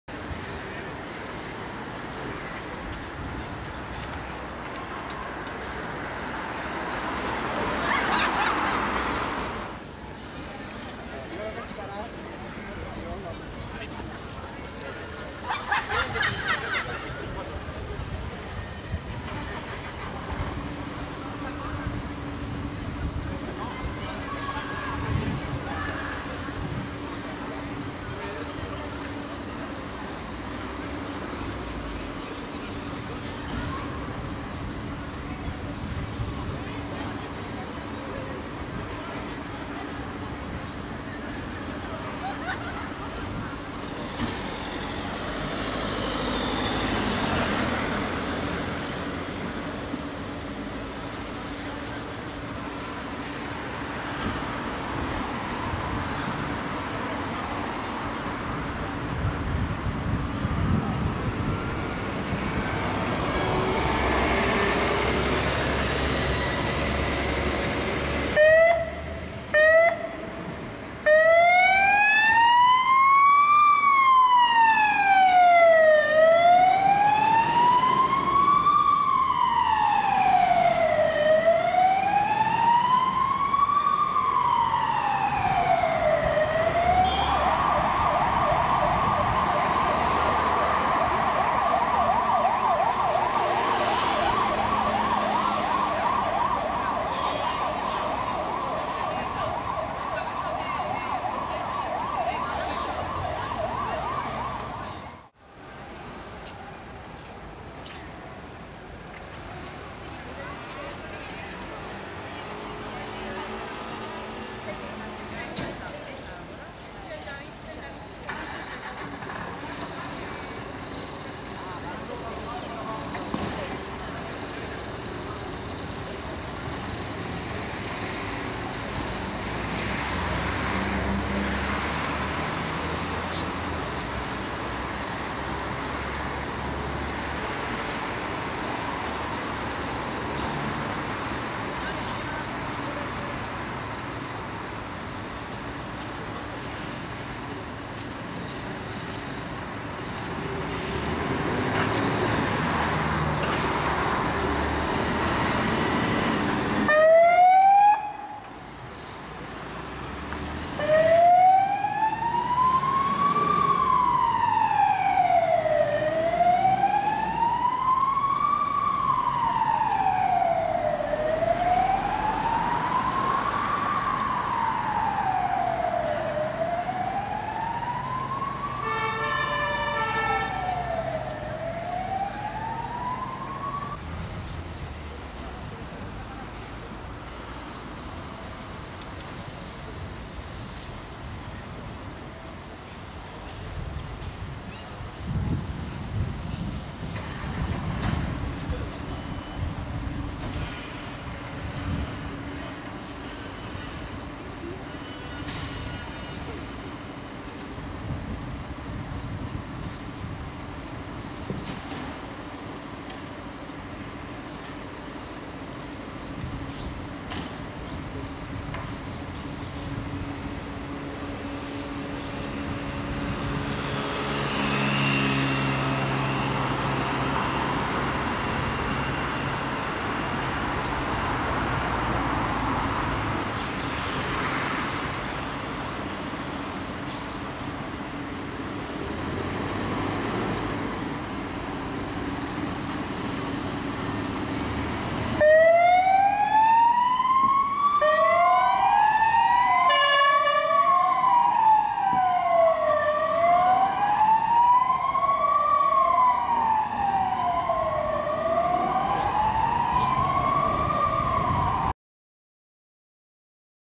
Barcelona Fire Department // Bombers de Barcelona Parc de Sant Andreu
Salida en urgencia de de los bomberos de Barcelona, parque de Sant Andreu.

Emergency calls for the Sant Andreu fire station of the Barcelona fire department. You can see pumpers B125 and B126 responding as well as rescue truck C10.

Einsatz für die Feuerwache Sant Andreu der Feuerwehr Barcelona. Zu sehen sind Ausfahrten für die Löschfahrzeuge B125 und B126 sowie Rüstwagen C10.

by Dirk Steinhardt - Barcelona, Spain / España - 11.2021